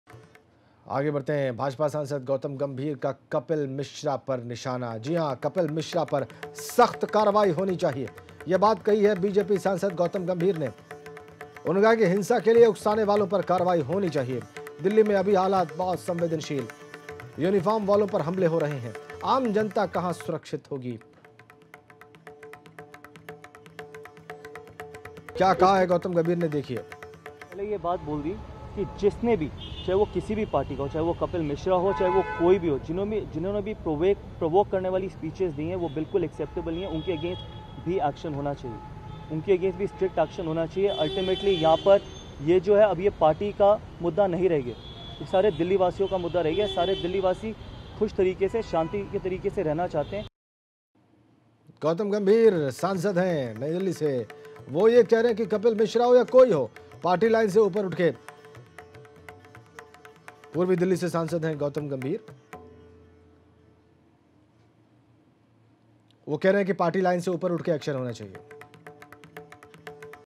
DELHI में अभी बेहद संवेदनशील हालात- Gambhir | ABP Ganga
भाजपा सांसद गौतम गंभीर का कपिल मिश्रा पर निशाना
'कपिल मिश्रा पर सख्त कार्रवाई होनी चाहिए'
'हिंसा के लिए उकसाने वालों पर कार्रवाई होनी चाहिए'
दिल्ली में अभी बेहद संवेदनशील हालात- गंभीर
'यूनिफॉर्म वालों पर हमले हो रहे, आम जनता कहां सुरक्षित होगी'